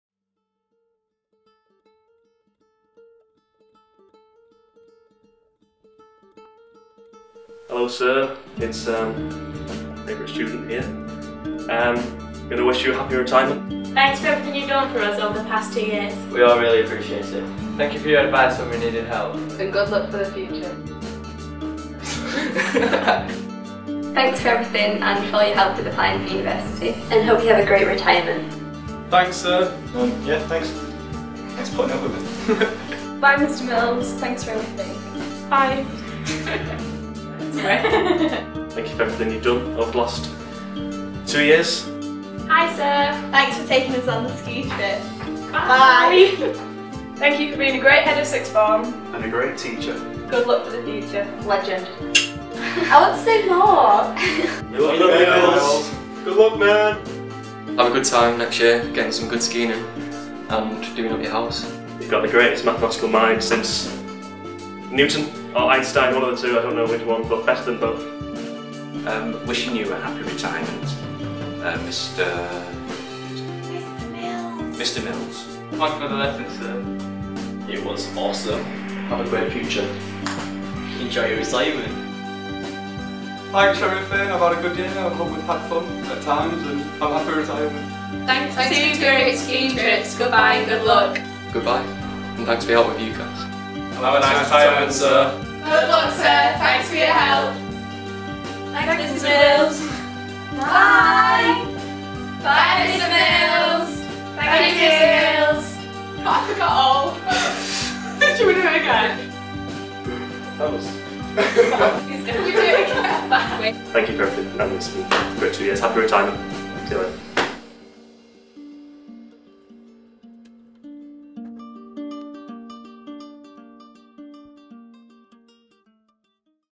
Goodbye Mr. Mills
Some messages and goodbyes for our departing Director of Sixth Form, Mr. Mills.